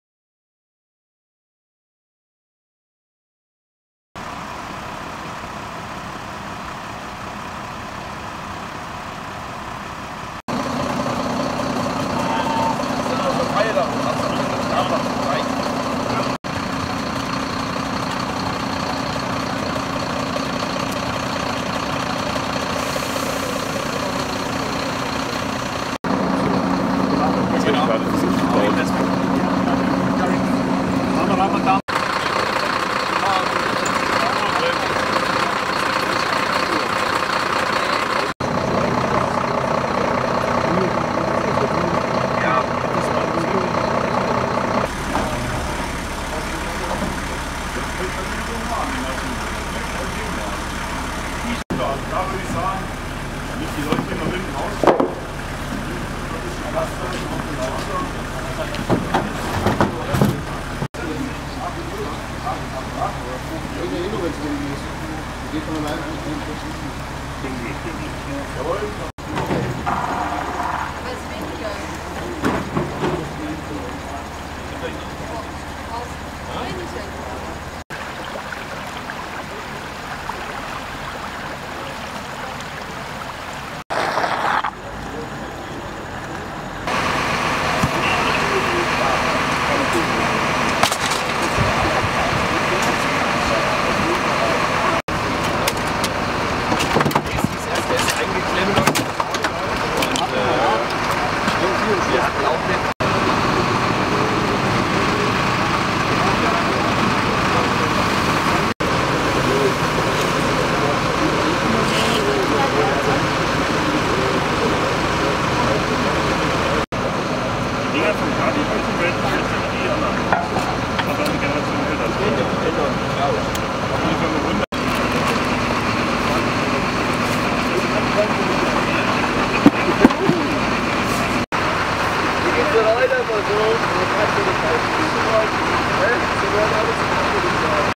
Viele Feuerwehreinsätze am Wochenende
Unfälle, Dauerregen und eine Buspanne hielt die Feuerwehren der Region das ganze Wochenende auf Trab.